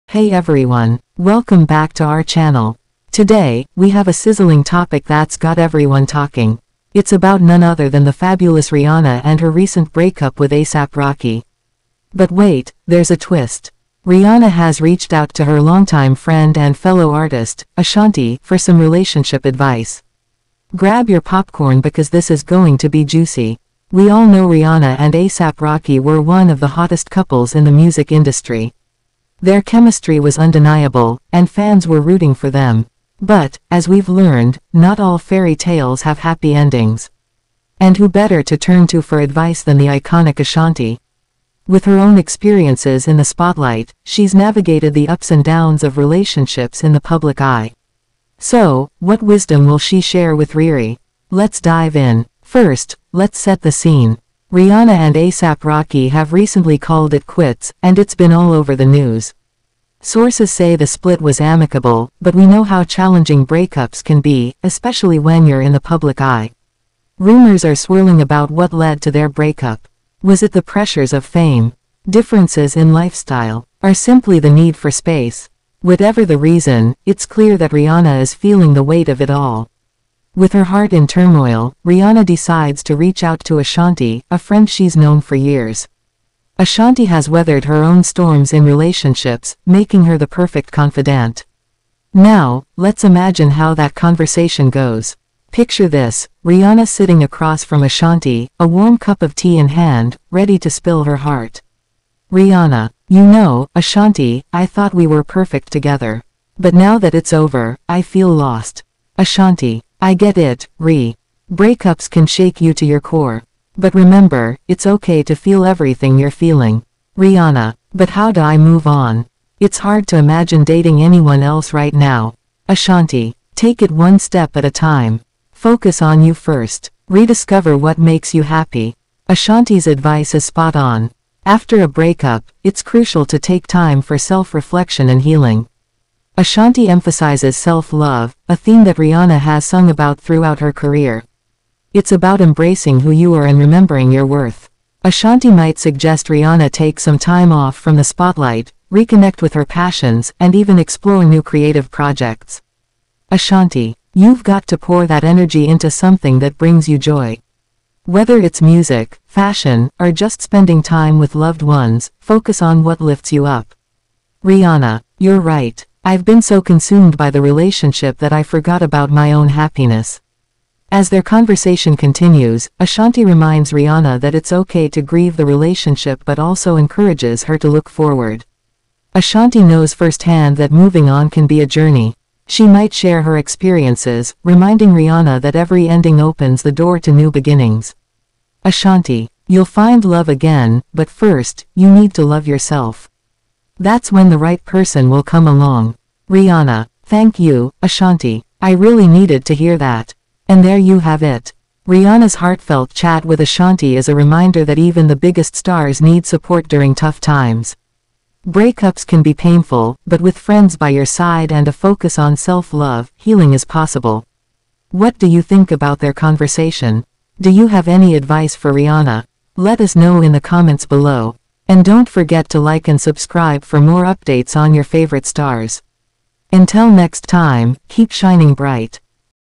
RIHANNA SEEKS RELATIONSHIP ADVICE FROM ASHANTI AFTER HER BREAK UP WITH ASAP ROCKY. click & see more🥺
Hey everyone! Today, we have a sizzling topic that’s got everyone talking. It’s about none other than the fabulous Rihanna and her recent breakup with A$AP Rocky. But wait—there’s a twist! Rihanna has reached out to her longtime friend and fellow artist, Ashanti, for some relationship advice. Grab your popcorn because this is going to be juicy!

We all know Rihanna and A$AP Rocky were one of the hottest couples in the music industry. Their chemistry was undeniable, and fans were rooting for them. But, as we’ve learned, not all fairy tales have happy endings.

And who better to turn to for advice than the iconic Ashanti? With her own experiences in the spotlight, she’s navigated the ups and downs of relationships in the public eye. So, what wisdom will she share with RiRi? Let’s dive in!

First, let’s set the scene. Rihanna and A$AP Rocky have recently called it quits, and it’s been all over the news. Sources say the split was amicable, but we know how challenging breakups can be, especially when you’re in the public eye.

Rumors are swirling about what led to their breakup. Was it the pressures of fame? Differences in lifestyle? Or simply the need for space? Whatever the reason, it’s clear that Rihanna is feeling the weight of it all.

With her heart in turmoil, Rihanna decides to reach out to Ashanti, a friend she’s known for years. Ashanti has weathered her own storms in relationships, making her the perfect confidante.

Now, let’s imagine how that conversation goes. Picture this: Rihanna sitting across from Ashanti, a warm cup of tea in hand, ready to spill her heart.

**Rihanna
You know, Ashanti, I thought we were perfect together. But now that it’s over, I feel lost.

**Ashanti
I get it, Ri. Breakups can shake you to your core. But remember, it’s okay to feel everything you’re feeling.

**Rihanna:**
But how do I move on? It’s hard to imagine dating anyone else right now.

**Ashanti:**
Take it one step at a time. Focus on you first. Rediscover what makes you happy!

Ashanti’s advice is spot on! After a breakup, it’s crucial to take time for self-reflection and healing.

Ashanti emphasizes self-love—a theme that Rihanna has sung about throughout her career. It’s about embracing who you are and remembering your worth.

Ashanti might suggest Rihanna take some time off from the spotlight, reconnect with her passions, and even explore new creative projects.

**Ashanti
You’ve got to pour that energy into something that brings you joy. Whether it’s music, fashion, or just spending time with loved ones, focus on what lifts you up.

**Rihanna:**
You’re right. I’ve been so consumed by the relationship that I forgot about my own happiness.

As their conversation continues, Ashanti reminds Rihanna that it’s okay to grieve the relationship but also encourages her to look forward.